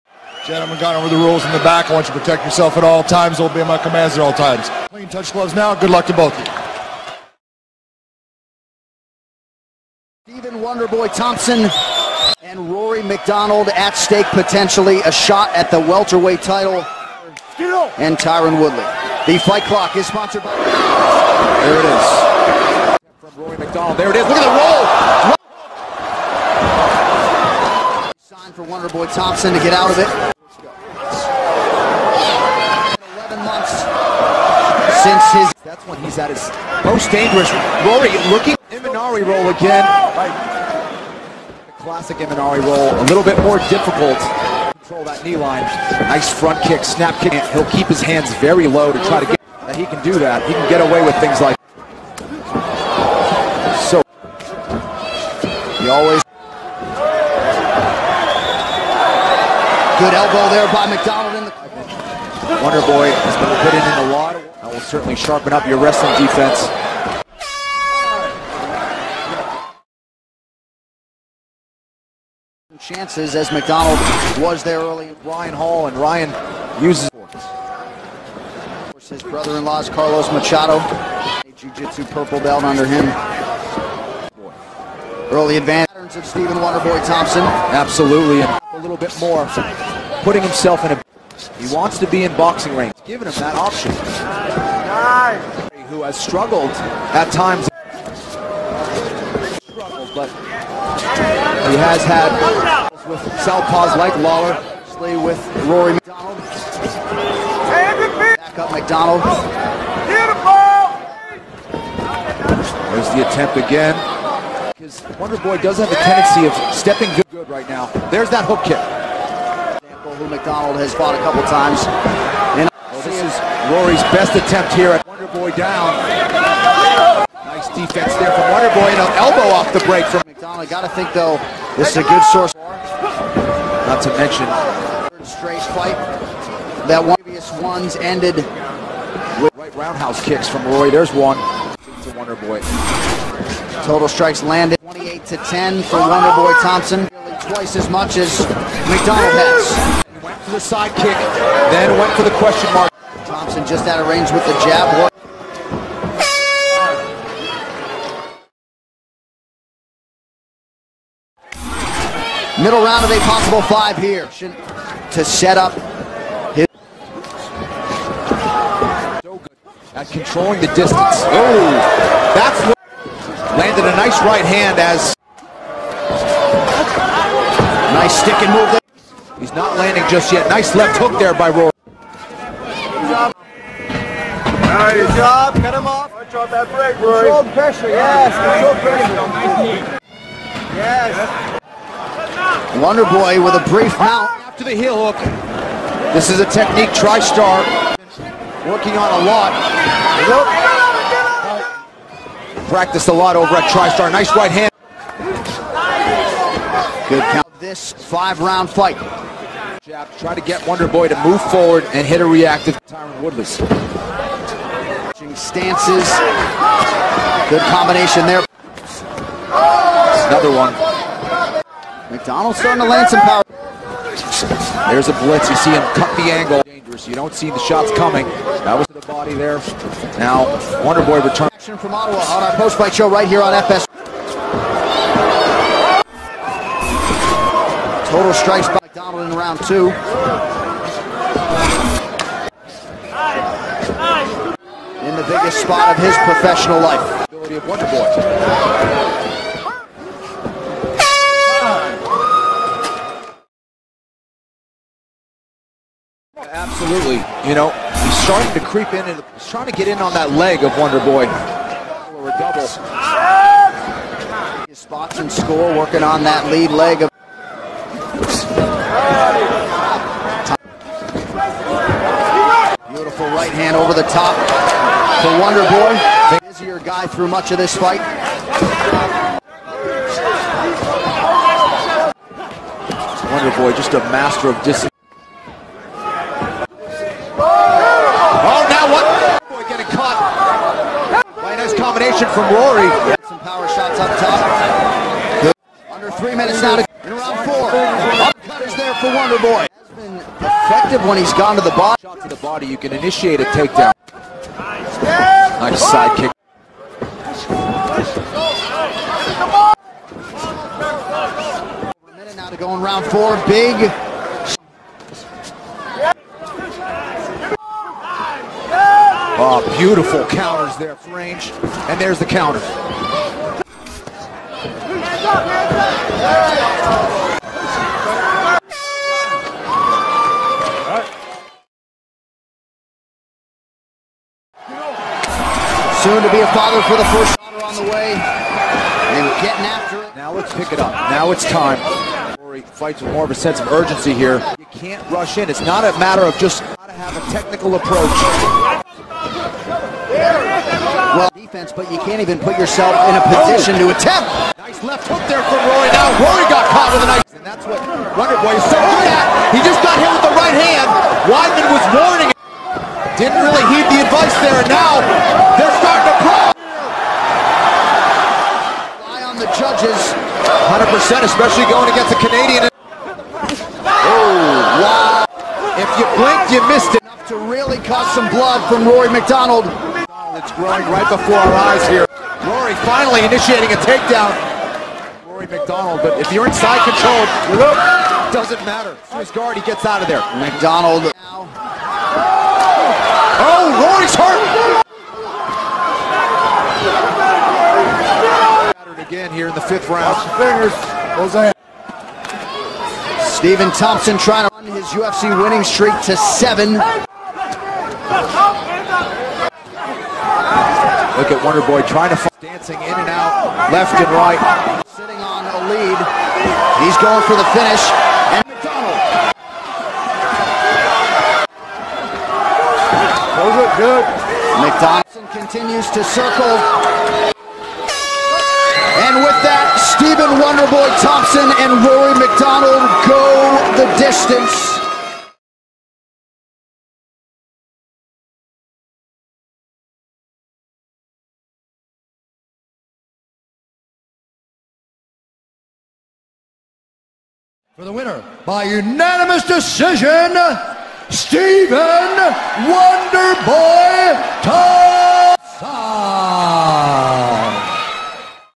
Stephen Thompson vs Rory MacDonald Highlights (Good FIGHT)
UFC Fight Night: MacDonald vs. Thompson (also known as UFC Fight Night 89) was a mixed martial arts event held on June 18, 2016, at TD Place Arena in Ottawa, Ontario, Canada.

The event was the first that the promotion hosted in Ottawa.

The card was headlined by a welterweight bout between former UFC Welterweight Championship challenger Rory MacDonald and five-time kickboxing world champion Stephen Thompson.

Thompson won the fight via unanimous decision (50–45, 50–45, and 48–47).